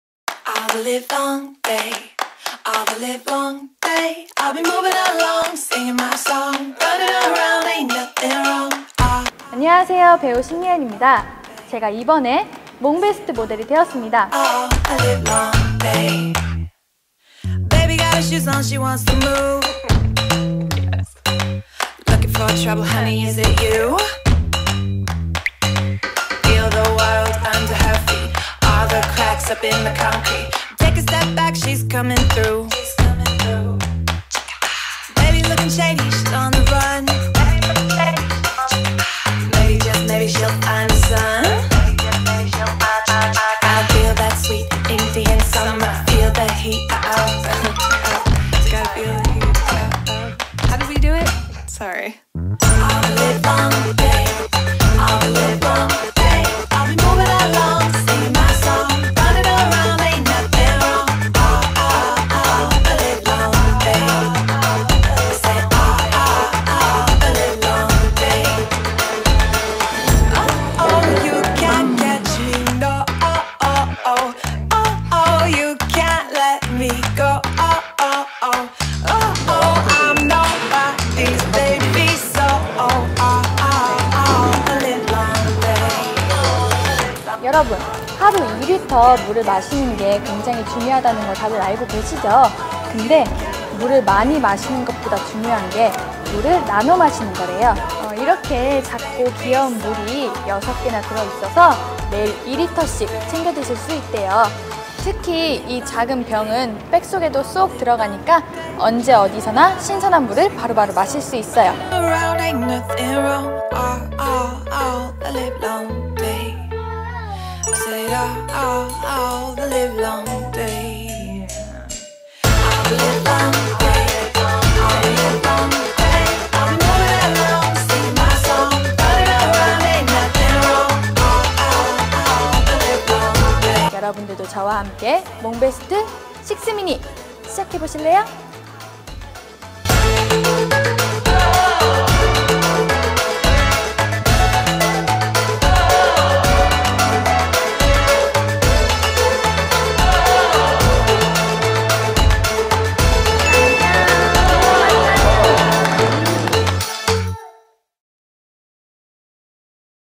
[몽베스트] TVCF 2020 메이킹 영상 ‘신예은'편
몽베스트 TV 광고 메이킹 영상입니다.

프리미엄 네츄럴 미네랄 워터, 몽베스트로
더 쉽고 간편하게 챙기세요 !

Make your Wish, MONTBEST